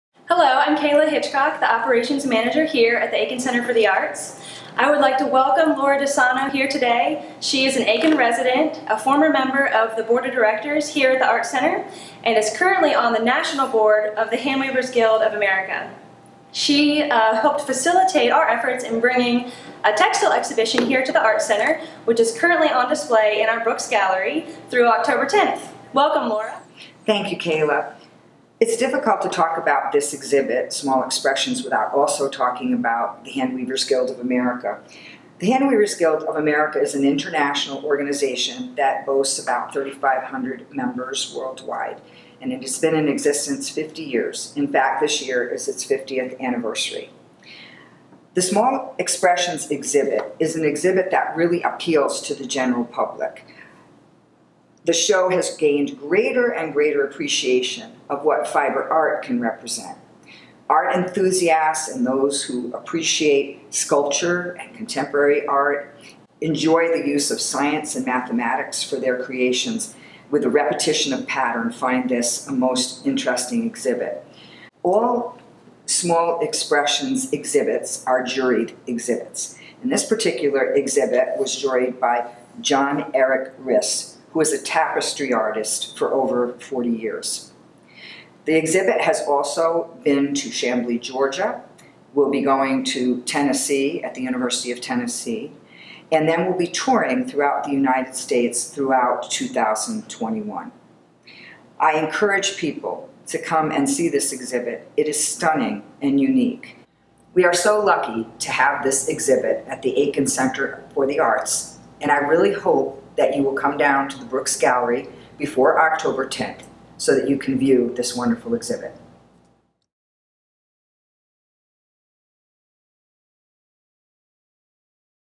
"Small Expressions" Fiber Art Exhibition by The Handweavers Guild of America
The Handweavers Guild of America, Inc. highlights contemporary, small-scale works created using fiber techniques in any media in their exhibition “Small Expressions”. This exhibition was featured in the Brooks Gallery from September 10 - October 25, 2019.
An annual juried exhibition showcasing contemporary small-scale works using fiber techniques in any media. Small in scale but visually compelling, the Handweavers Guild of America, Inc.’s (HGA) Small Expressions exhibit features work that speaks to the intricacy of expression, intimacy of design, thoughtful communication, and visual excitement. The artists in this exhibit create a big impact through small pieces.